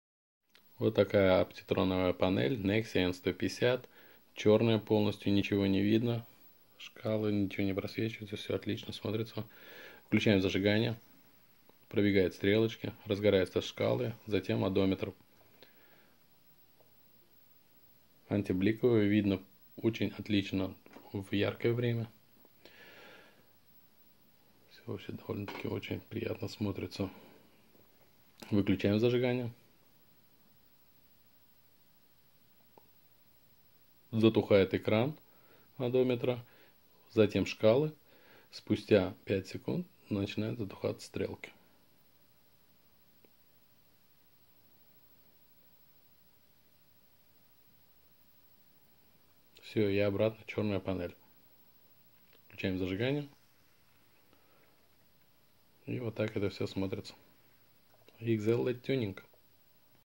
Подсветка прибора Nexia тюнинг под оптитрон от IgZeLLedTuninG
Смотрим результат проделанной работы по тюнингу приборной панели. Очередная приборка была изготовлена для автомобиля NEXIA.  То самое чувство, когда панель приборов в твоей NEXIA выглядит иначе😎👍💪
🔸 Оптитронная накладка
🔸 Разделение поэтапности включения подсветки. Отдельная светодиодная плата для точечной подсветки цифр и шкалы, отдельная подсветка для стрелок, отдельная подсветка для жк экрана одометра.
🔸Инверсия экрана
🔸Установлен 2х канальный блок плавного розжига и гашения
🔸Темная тонировка стекла
Всё это вы получите заказав оптитронную панель приборов для
Приборку нам передаёте на переделку и в итоге получаете совершенно новые впечатления сидя за рулём своего автомобиля. .
IgZeLLedTuninG - Комфортный свет в Вашем автомобиле!